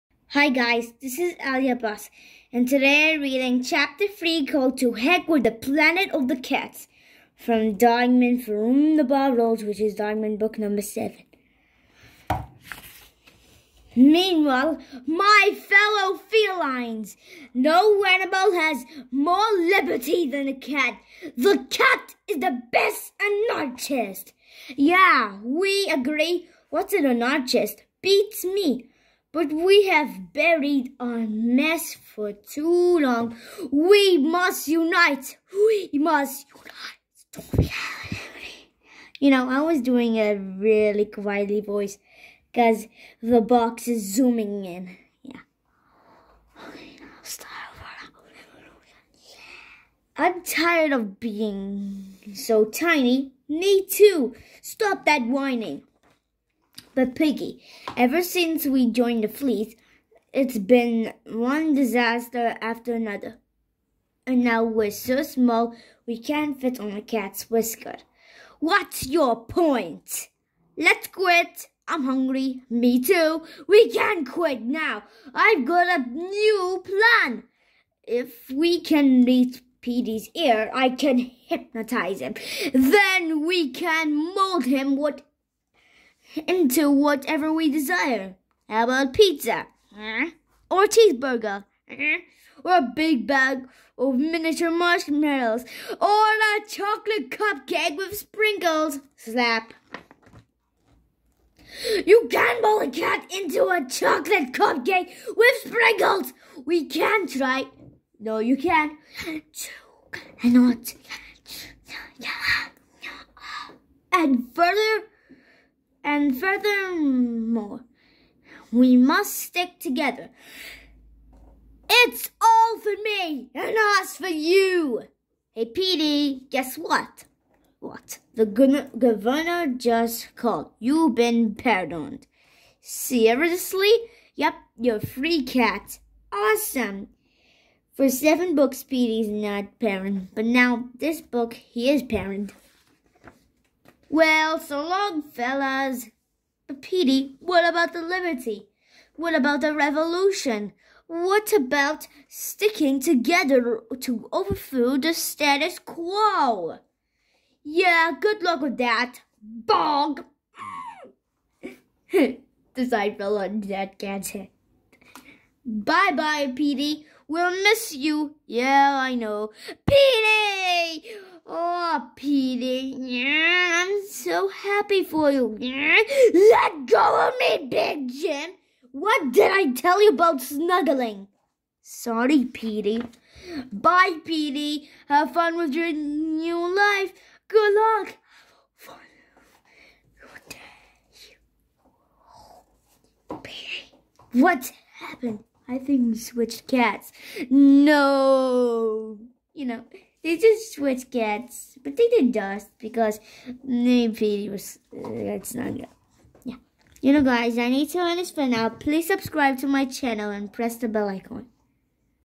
To Heck with the Planet of the Cats - Dog Man: For Whom The Ball Rolls | Book 7 Chapter 3
All the cats get revenge. And on Petey's whisker, there is the fleas, they got revenge but only Piggy. Fortunately, Petey been pardoned.

Supa Buddies Cat Kid, The Bark Knight and Lightning Dude.